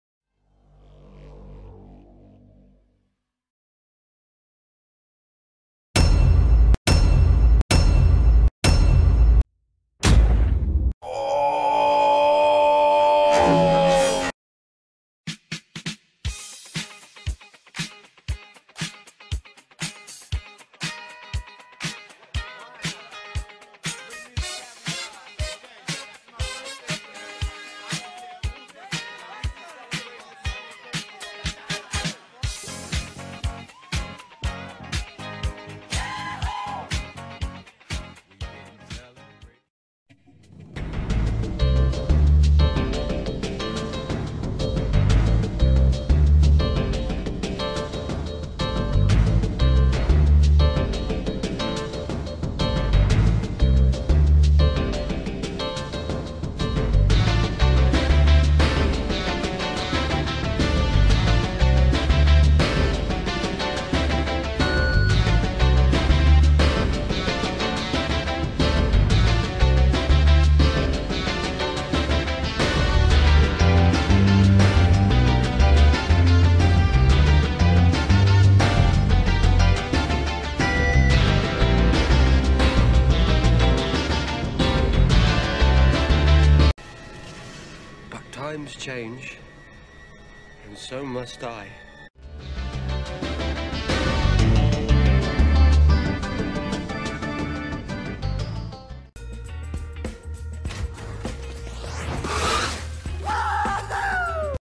100K Subscribers?! (Q&A Coming!)
I dun did da thing, yet still surprised on the how.
(A short little video to thank you all, and remember to ask questions for the Q&A! )